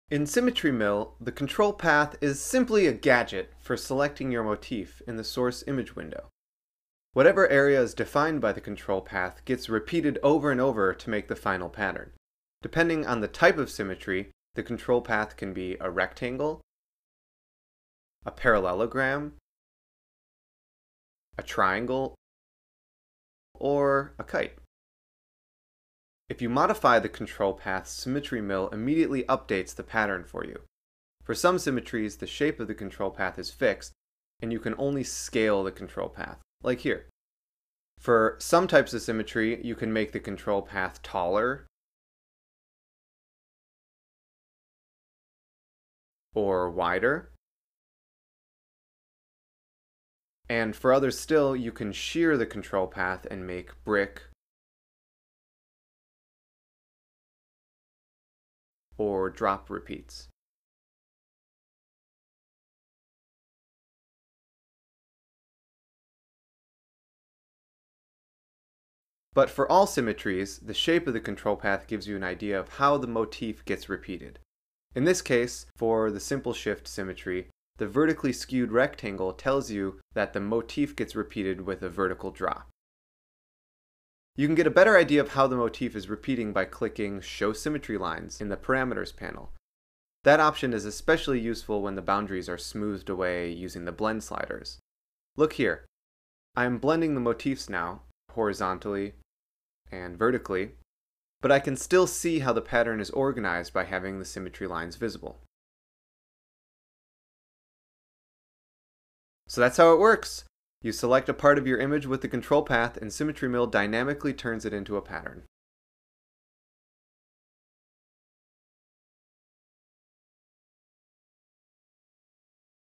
The control path in Artlandia SymmetryMill: Choosing the motif for your seamless repeating patterns
The control path is the most important tool in SymmetryMill. Moving and modifying the control path produces an incredible variety of repeating patterns. This short tutorial gets you up to speed with the basics.